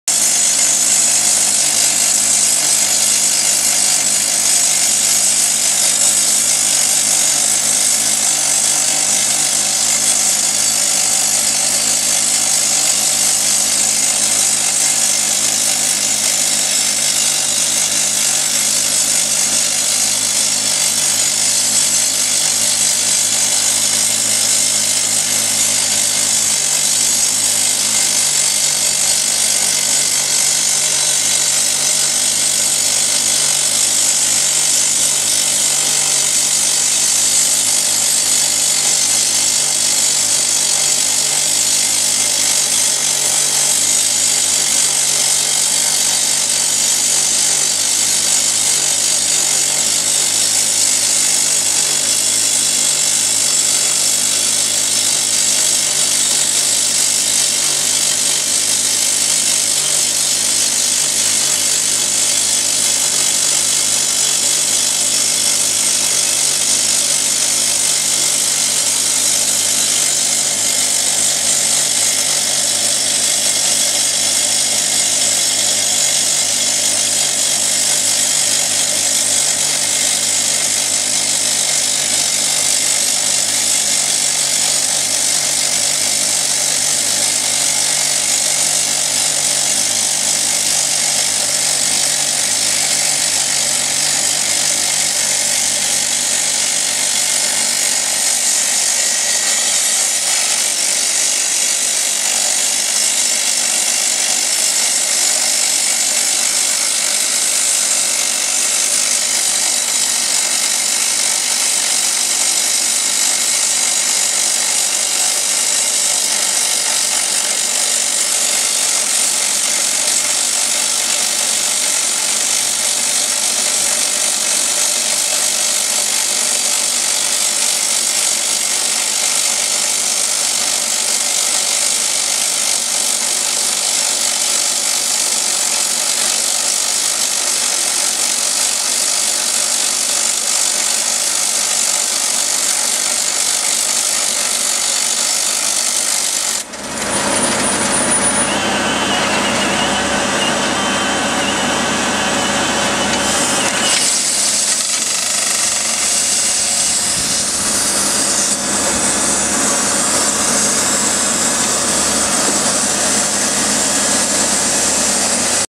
DRILLCOM RD32 ROCK DRILL(2)
Drilling 70mm holes with button bit to hard grey granite. Hole depth 2500mm.